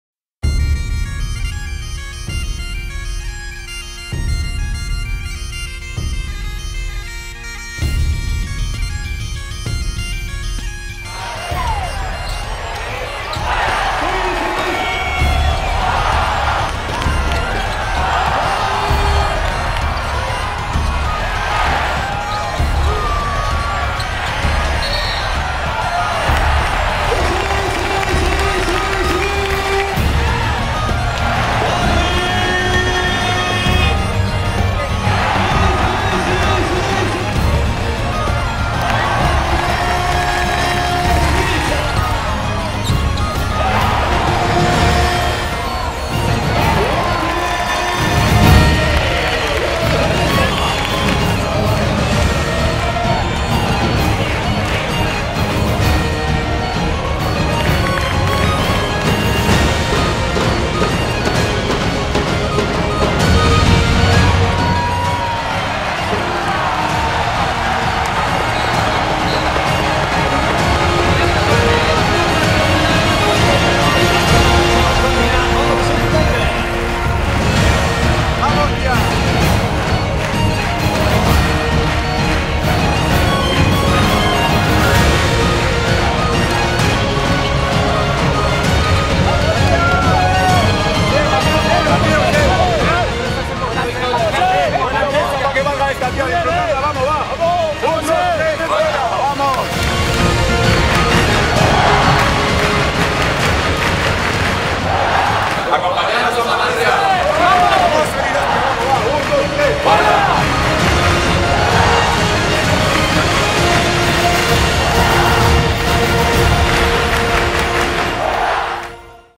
Juntos lo conseguimos, ¡juntos lo conseguiremos!
Dos partidos, dos victorias, una comunión especial entre la grada y la cancha. Una prueba irrefutable de que juntos lo conseguiremos, ¡acompáñanos a Manresa!